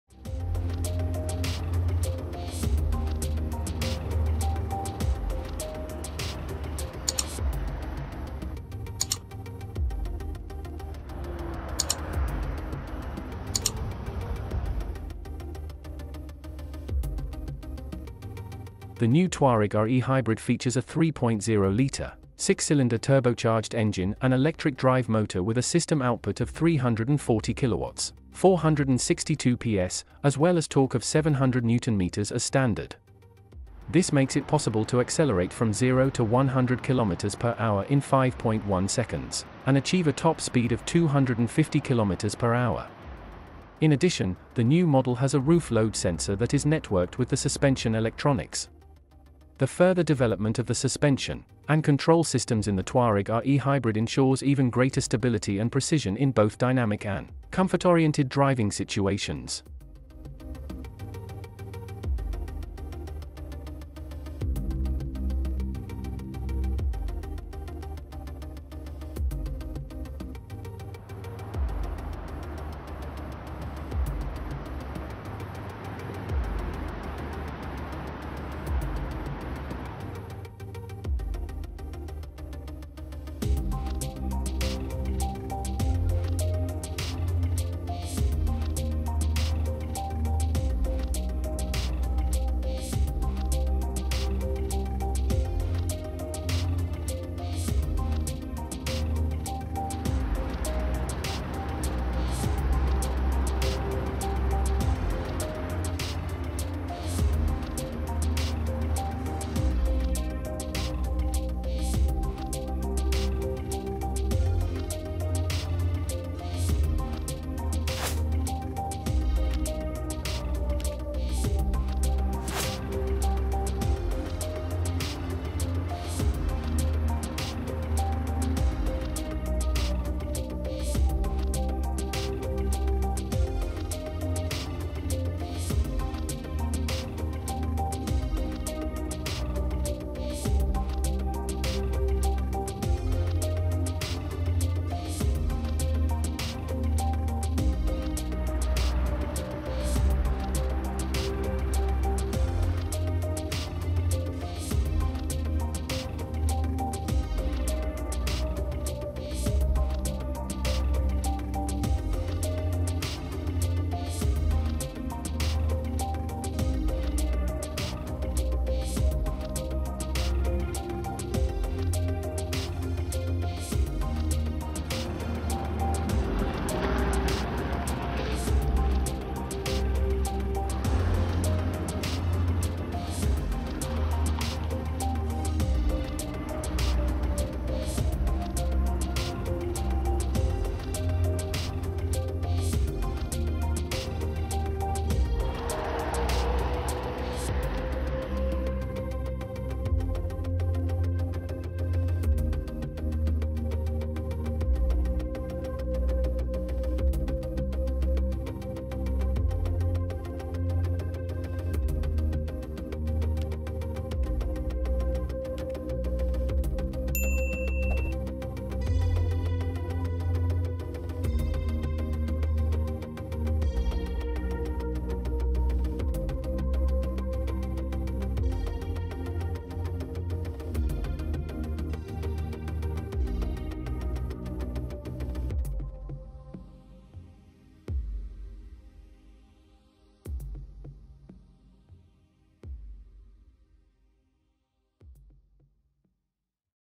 Volkswagen Touareg R eHybrid with 3 0 litre, six cylinder
Touareg R eHybrid
The new Touareg R eHybrid features a 3.0-litre, six-cylinder turbocharged engine and electric drive motor with a system output of 340 kW (462 PS) as well as torque of 700 Nm as standard. This makes it possible to accelerate from 0 to 100 km/h in 5.1 seconds and achieve a top speed of 250 km/h. In addition, the new model has a roof load sensor that is networked with the suspension electronics. The further development of the suspension and control systems in the Touareg R eHybrid ensures even greater stability and precision in both dynamic and comfort-oriented driving situations.

Numerous innovations – standard and optional. The standard highlights of the premium model include the redesigned, even more intuitive Innovision Cockpit with a central touchscreen (display diagonal: 38 centimetres, or 15 inches) and the new IQ.LIGHT HD matrix headlights. Their more than 38,000 interactive LEDs project a light carpet exactly onto the driving lane and illuminate the road more precisely than ever before. A further optional high-tech feature is available for the Touareg R eHybrid: Park Assist Pro prepared for remotely controlled parking. It enables control of the process of driving in and out of parking spaces from a smartphone (via Volkswagen App) and can also be used in very tight parking spaces.

In addition, the new Touareg R eHybrid stands out with the following standard features:

Differentiated design in Black Style for a sporty look (including R-specific front bumper inserts, black tailpipes and glossy black roof rails)
Blue brake calipers with R logo
VW logo in the rear illuminated in red
R logo projection via exterior mirror housings as surround lighting
Blue accents in the interior
Leather seats in the Vienna design with Crystal Grey stitching (sustainable Puglia leather with blue stitching optional)
21-inch “Leeds” alloy wheels in combination with air suspension
Sill panel mouldings with illuminated R logo
Vehicle key with leather-covered cap and R logo
Panoramic sliding sunroof
4-zone air conditioning system
The flagship model can alternatively be fitted with additional 21-inch and 22-inch alloy wheels on request. In addition to the traditional Lapiz Blue metallic R colour, the Touareg R eHybrid can now be ordered in the optional matt paintwork, Silicon Grey matt.